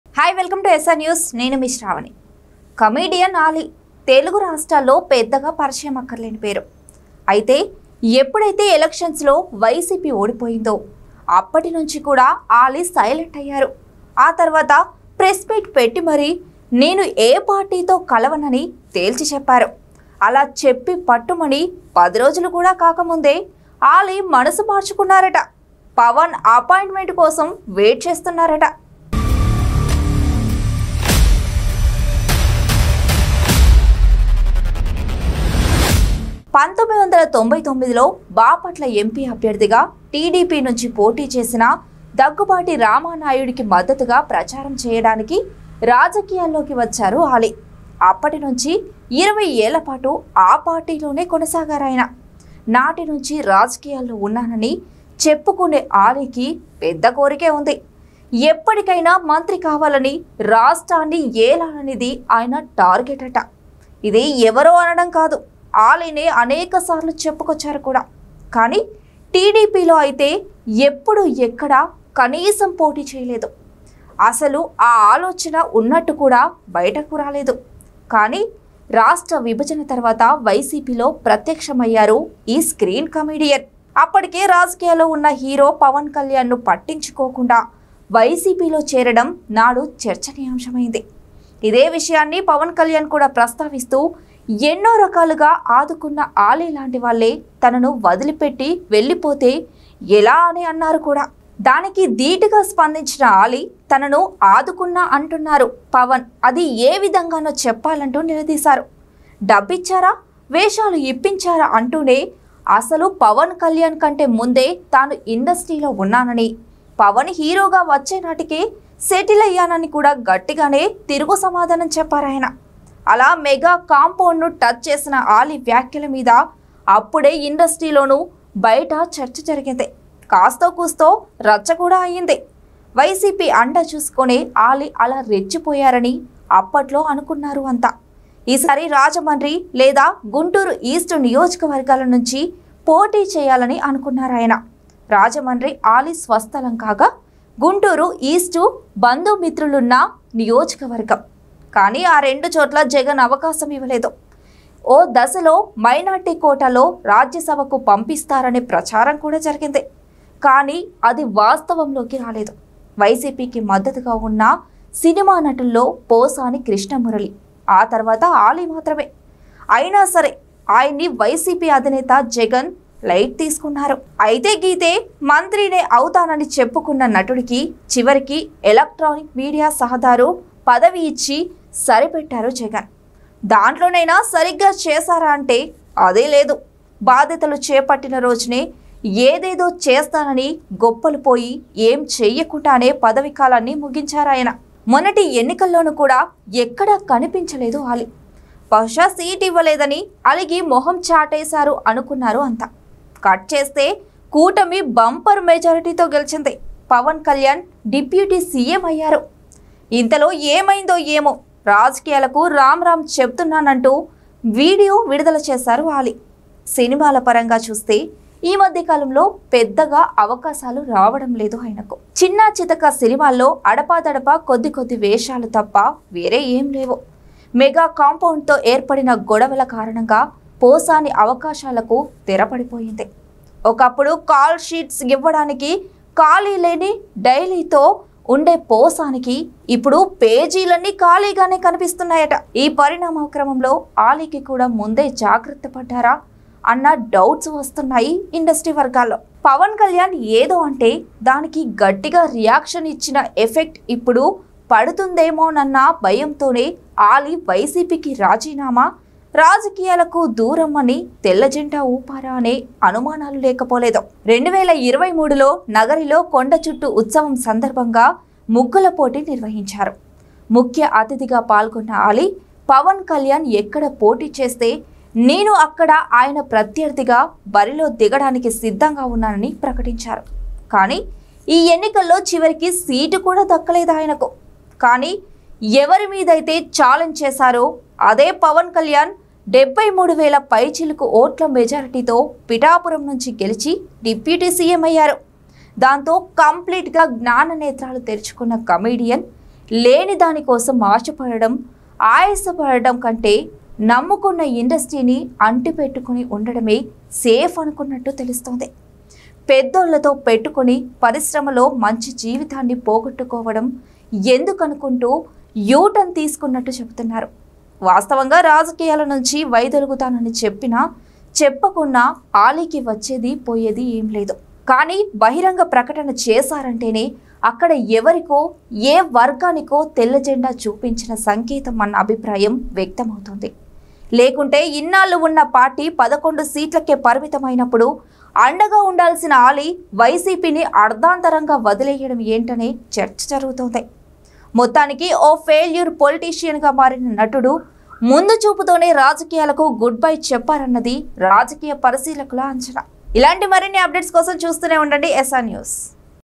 Comedian Ali Is Waiting For Pawan Kalyan Appointment | Ali Resigns YCP | AP Politics | Janasena | SR
Comedian Ali Is Waiting For Pawan Kalyan Appointment | Ali Resigns YCP | AP Politics | Janasena | SR News